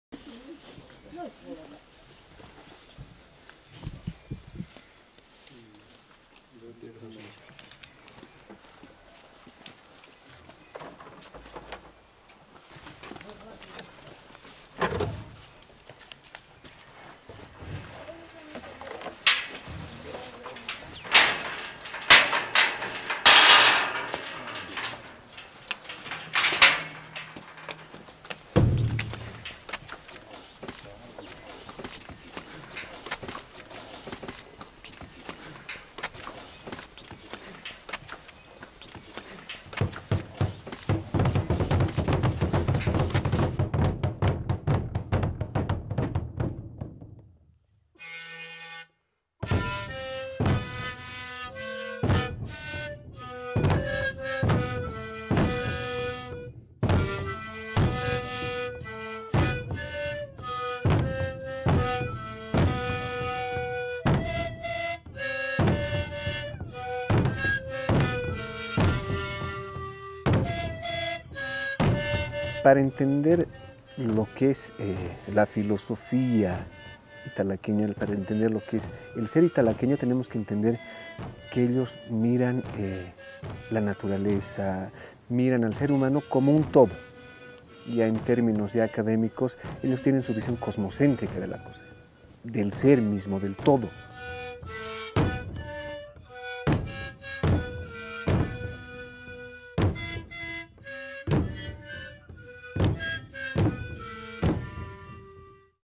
BOL247 - The Italaque Sessions 1 - Boris Bernal
Italaque, a small village north from La Paz, Bolivia. Their traditional sacred music the Sicuris and Boris Bernal talking about love to Nature.